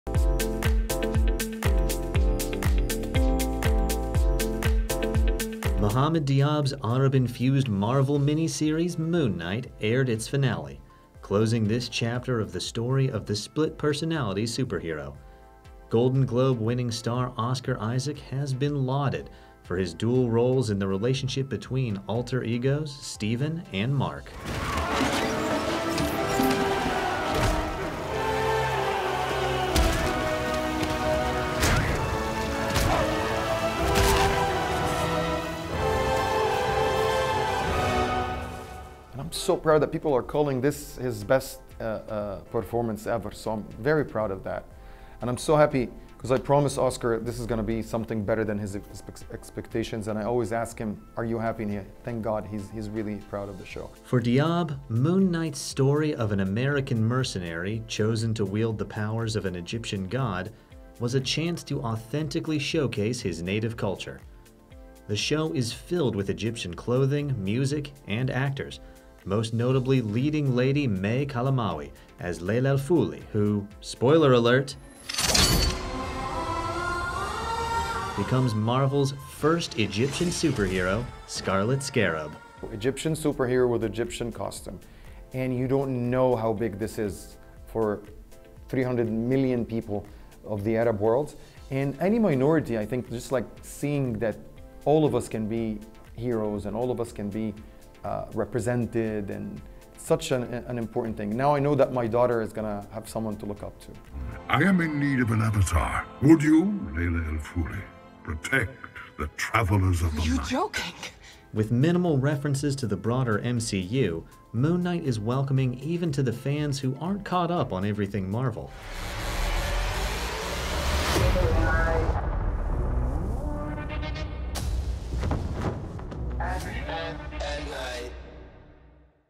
Marvel's MoonKnight - Interview with Mohamed Diab
Marvel's most diverse series "Moon Knight" just finished its first season. With an abundance of Egyptian actors - most notably female lead May Calamawy - Egyptian settings, music and costumes, this truly international production also stars Oscar Isaac and Ethan Hawke. The HFPA's Raffi Boghosian spoke with director and executive producer Mohamed Diab, himself a filmmaker from Egypt.